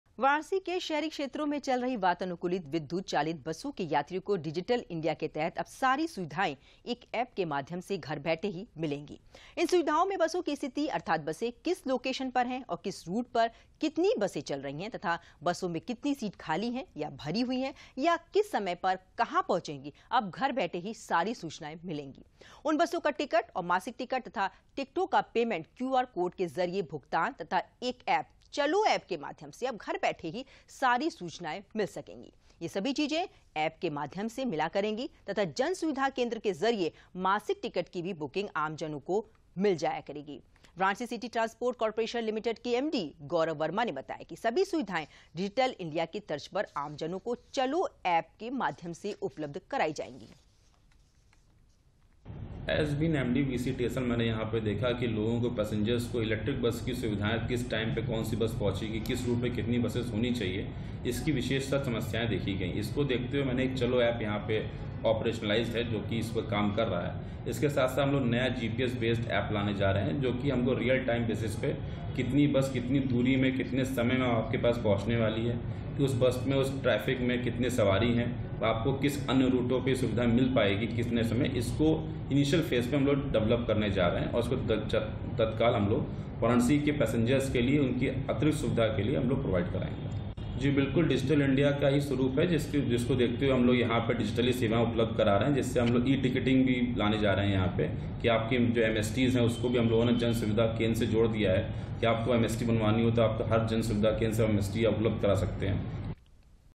Varanasi : एक ऐप के माध्यम से घर बैठे मिलेगी विद्युत चालित बसों की सारी जानकारी
वाराणसी के शहरी क्षेत्रों में चल रही वातानुकूलित विद्युत चालित बसों के यात्रियों को डिजिटल इंडिया के तहत अब सारी सुविधाएं एक ऐप के माध्यम से घर बैठे ही मिलेंगी।  इन सुविधाओं में बसों की स्थिति अर्थात बसें किस लोकेशन पर हैं और किस रूट पर कितनी बसें चल रही हैं तथा बसों में कितनी सीटें खाली हैं या भरी हुई है या किस समय पर कहां पहुंचेगी अब घर बैठे ही सारी सूचनाएं मिलेंगी । उन बसों का टिकट और मासिक टिकट तथा टिकटों का पेमेंट क्यूआर कोड के जरिए भुगतान तथा एक ऐप चलो ऐप के माध्यम से अब घर बैठे ही सारी सूचनाएं मिलेंगी, यह सभी चीजें ऐप के माध्यम से मिला करेंगी तथा जन सुविधा केंद्र के जरिए मासिक टिकट की भी बुकिंग आम जनों को मिल जाया करेगा।  वाराणसी सिटी ट्रांसपोर्ट कारपोरेशन लिमिटेड के एमडी गौरव वर्मा ने बताया कि सभी सुविधाएं डिजिटल इंडिया के तर्ज पर आम जनों को चलो ऐप के माध्यम से उपलब्ध कराया जाएगा ।